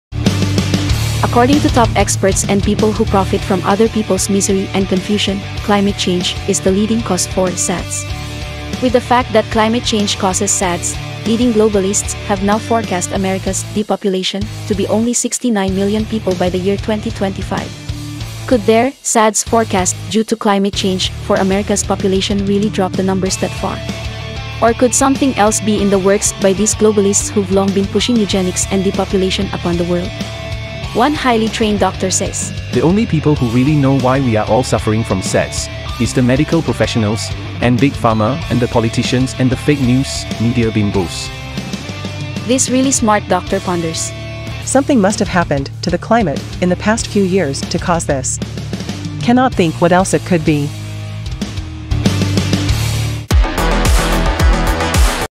Depopulation 69 Million, Climate Change SADS Part 2
According to top experts and people who profit from other people's misery and confusion, ‘climate change‘ is the leading cause for ‘SADS‘. With the fact that climate change causes SADS, Leading Globalists have now forecast America’s ‘depopulation‘, to be only 69 million people by the year 2025. Could their SADS forecast due to climate change‘ for America’s population really drop the numbers that far, or could something else be in the works by these globalists who’ve long been pushing eugenics and depopulation upon the world?

One highly trained doctor says… “The only people who really know why we are all suffering from sads, is the medical profession, and ‘big pharma‘ and the politicians and the fake news media bimbos.”

This really smart doctor ponders… “Something must have happened, to the climate, in the past few years to cause this….. cannot think what else it could be.”